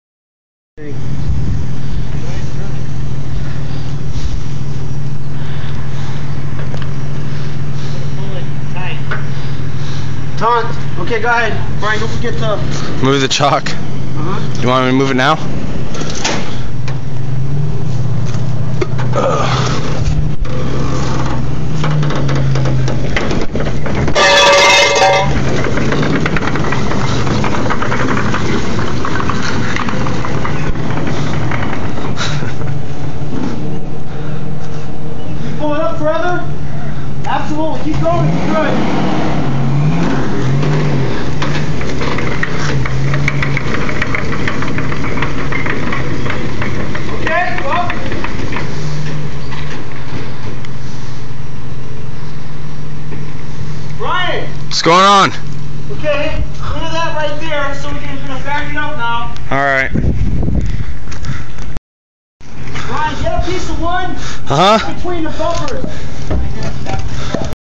WW2 U.S. Half Track Project
1942 M-1 Half track. Restoration in progress. New brakes, new tracks, new transmission, NOS White engine, custom modifications.